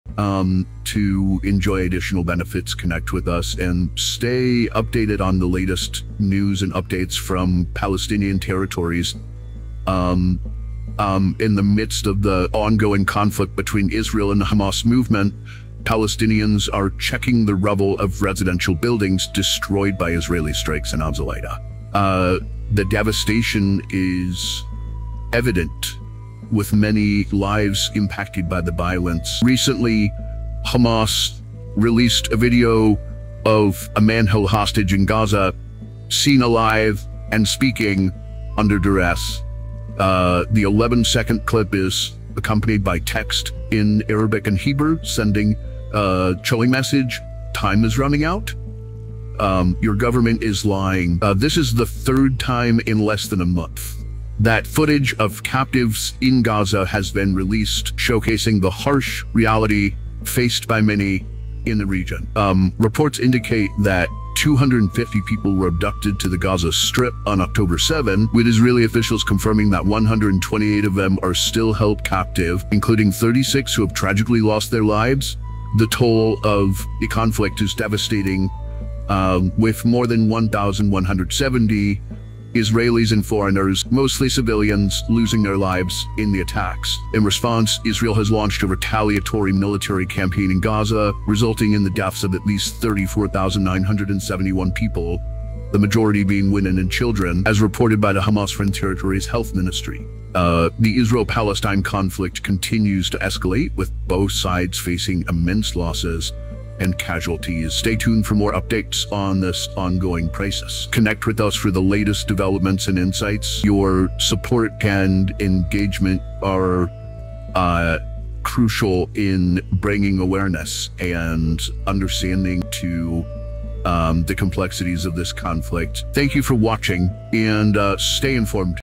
Tensions Rise in Palestinian Territories: Latest Updates on Israel-Hamas Conflict
YouTube video explore latest updates ongoing conflict Israel Hamas movement Palestinian Territories Palestinians are seen checking rubble residential building destroyed Israeli strike Al-Zawayda Ezzedine al-Qassam Brigades Hamas armed wing released video man held hostage Gaza hostage seen speaking duress message Arabic Hebrew stating Time is running out government is lying is third video less month showing captives held Gaza Israeli officials reporting still held captive including are dead following attack resulted deaths more Israelis foreigners retaliatory military campaign Gaza has resulted least deaths most being women children according Hamas-run territory health ministry Stay tuned learn more escalating tensions Israel-Palestine conflict